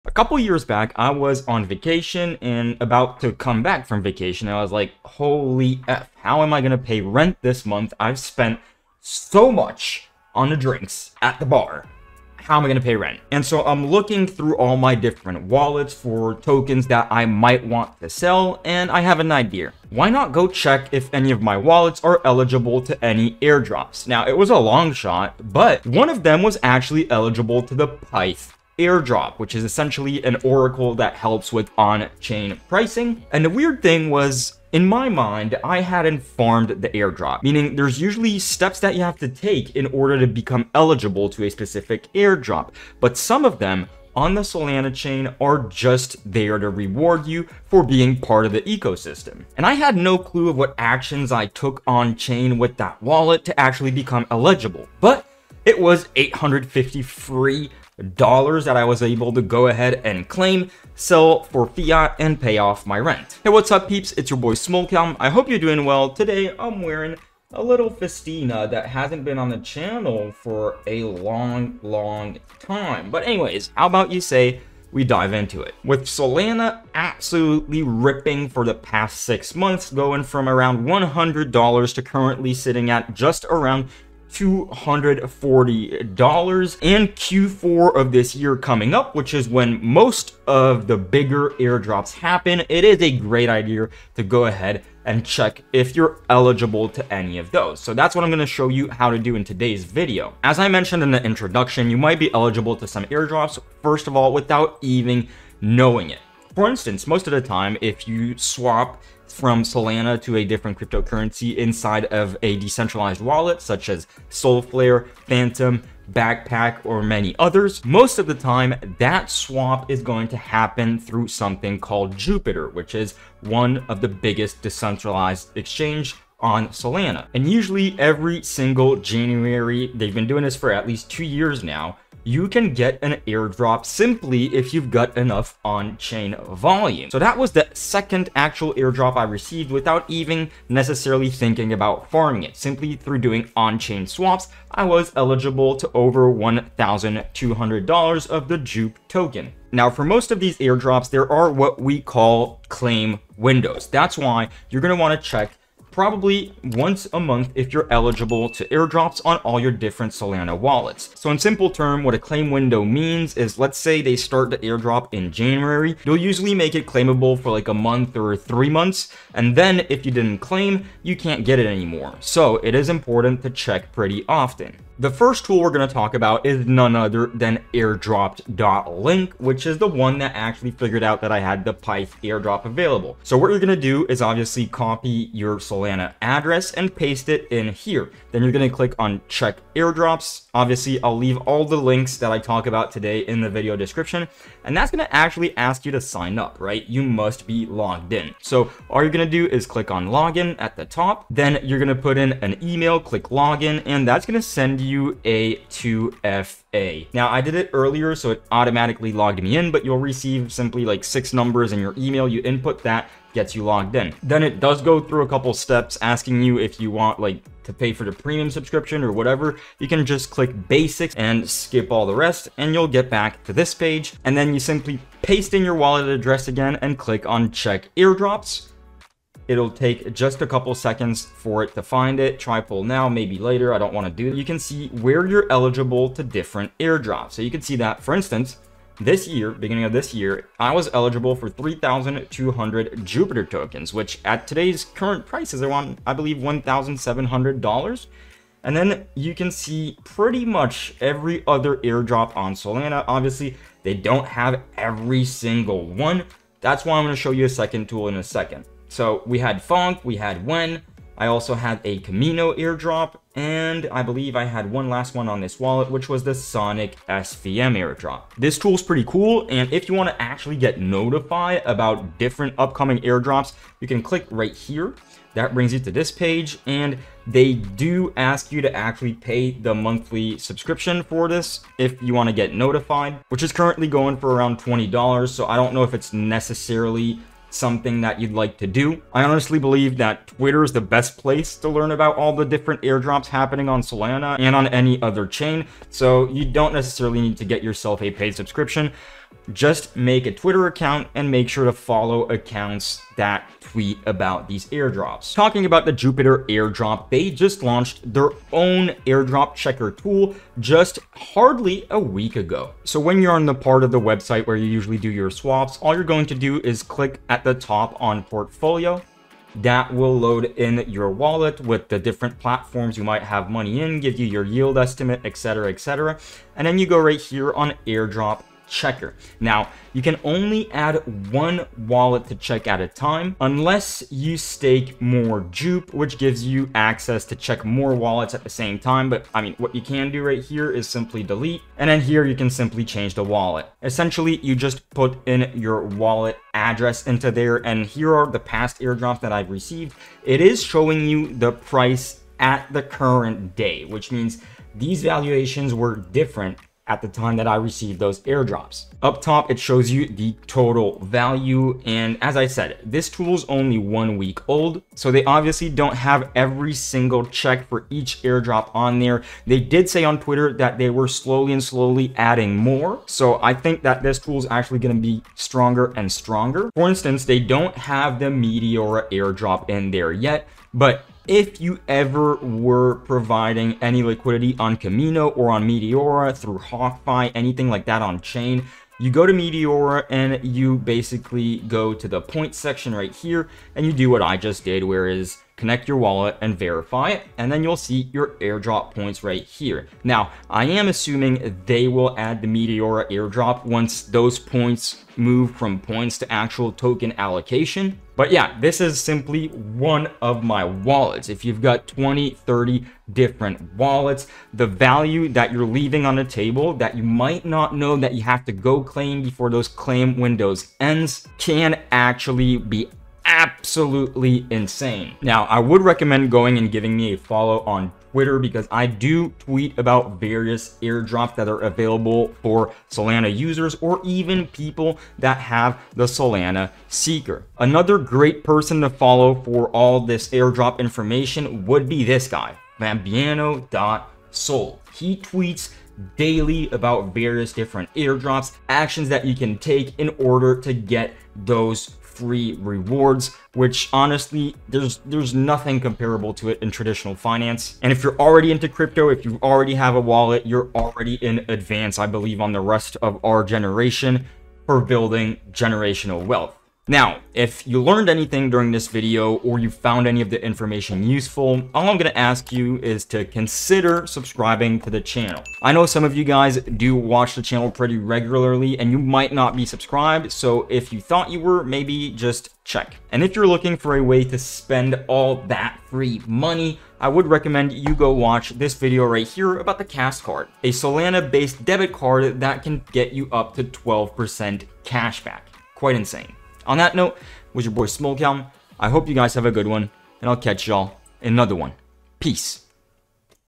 How To Check Wallets For Solana Airdrops (FULL GUIDE)
Solana Airdrops can be one of the most lucrative freebies in the world of cryptocurrency, you migh be eligible to some without even knowing it, so in this video, I show and explain how to check your wallets, what claim windows are, and why you should care!

If you'd like to send me a little gift my Seeker ID is: smoklm.skr